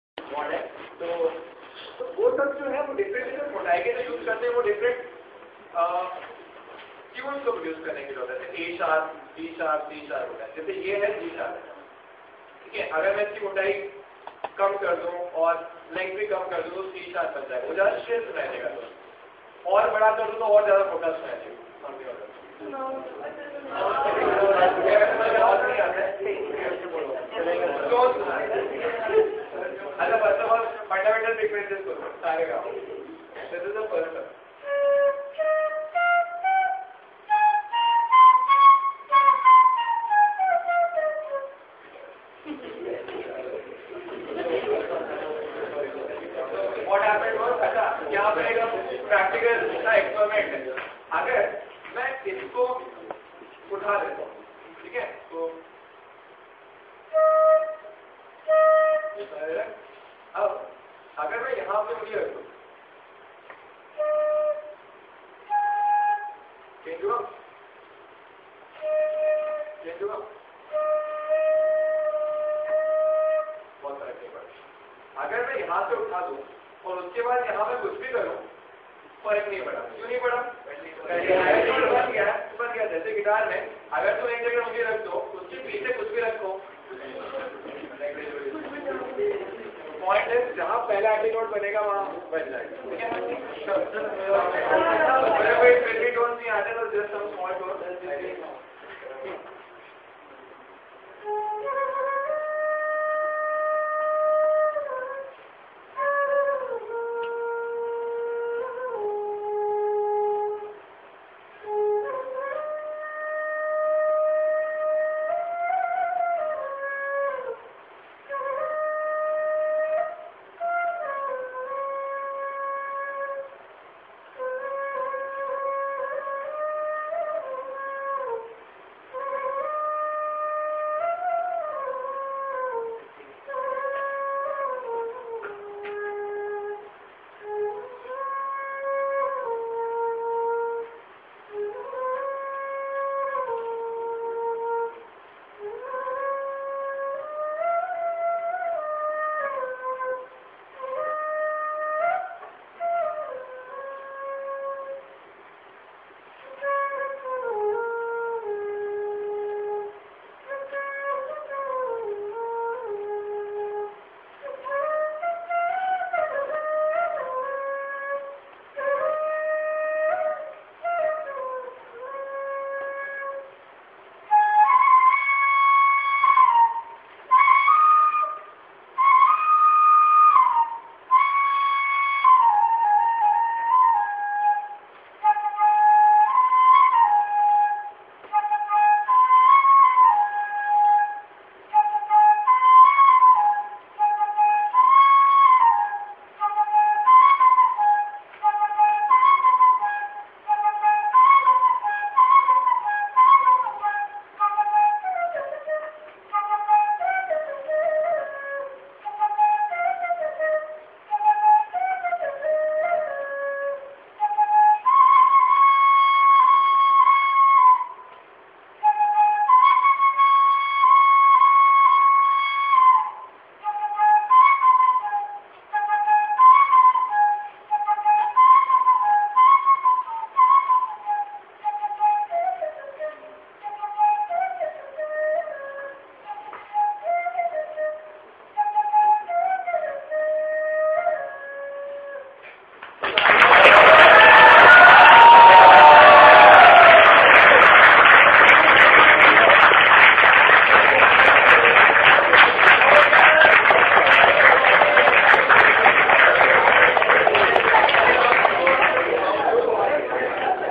Flu_e Mechanics
Our physics teacher, Vamsi Sir explaining the working of closed organ pipes. And one another thing, you'll find something cool at 1:40.